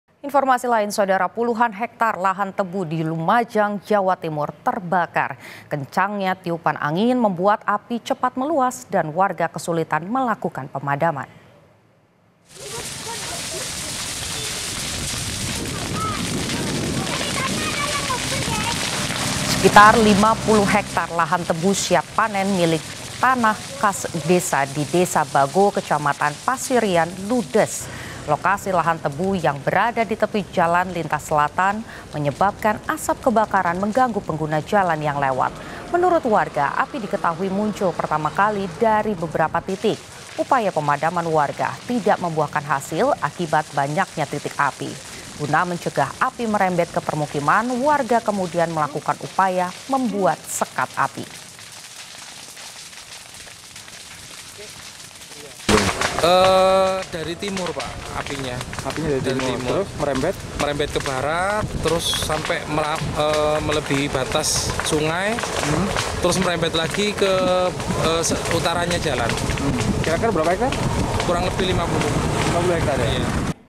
Api Muncul di Sejumlah Titik, 50 Hektare Lahan Tebu di Lumajang Jatim Terbakar Hebat!
KOMPAS.TV - Puluhan hektare lahan tebu di Lumajang, Jawa Timur, terbakar.

Kencang tiupan angin, membuat api cepat meluas dan warga kesulitan melakukan pemadaman.

Sekitar 50 hektare lahan tebu siap panen, milik tanah kas desa di Desa Bago, Kecamatan Pasirian, terbakar.

Lokasi lahan tebu yang berada di tepi Jalan Lintas Selatan, menyebabkan asap kebakaran mengganggu pengguna jalan yang lewat.

Menurut warga, api diketahui muncul pertama kali dari beberapa titik.

Baca Juga 13 Mobil Damkar Bantu Padamkan Kebakaran Tempat Usaha Percetakan di Tangsel! di